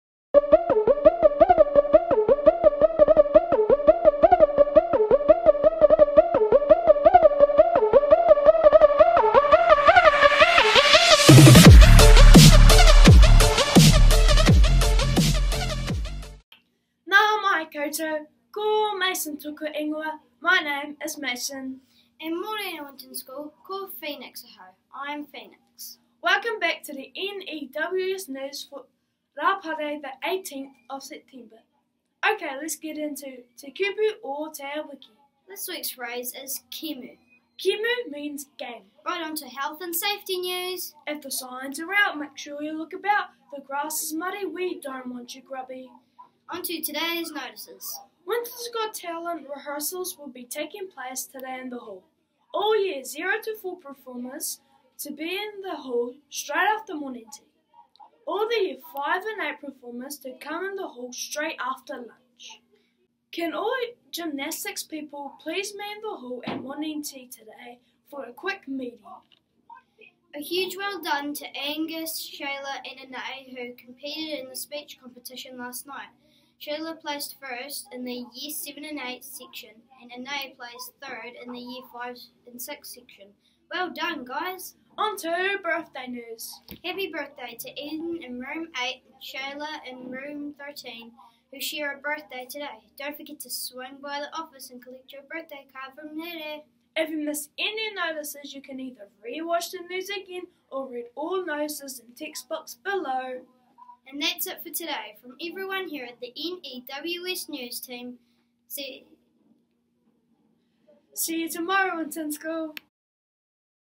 Winton School News 18.9.19
Nau mai koutou
Ko Anais toku ingoa
My name is Anais. //

And mo’rena WINTON SCHOOL,
KO  Ryan ahau.  I’m Ryan

Welcome to the N.E.W.S. news for Raapa the 18th of September!.

OK let’s get into:
Te Kupu o  te wiki… //

This week’s phrase is:  kēmu

kēmu  means game.

Right onto Health and Safety news:

If the signs are out make sure you look about, the grass is muddy we don’t want you grubby!

Onto todays notices…

Winton’s Got Talent Rehearsals will be taking place today in the hall. All Year 0-4 performers to be in the hall straight after morning tea. All Year 5-8 performers to come to the hall straight after lunch.

Can all gymnastics people please meet in the hall at morning tea today for a quick meeting.

A huge well done to the Angus, Shayla and Anais who competed in the speech competition last night. Shayla placed 1st in the Year 7 and 8 section and Anais placed third in the Year 5 and 6 section. Well done guys!

Onto Birthday News…

Happy birthday to Eden in Room 8 and Shayla in Rom 13 who share a birthday today. Don’t forget to swing by the office and collect your birthday cards from RERE.

if you missed any notices you can play the NEWS again or read all the notices in the text box below. ]

And that’s it for today. From everyone here at the N.E.W.S. news team:

See you tomorrow Winton School  ( BOTH SAY Wave)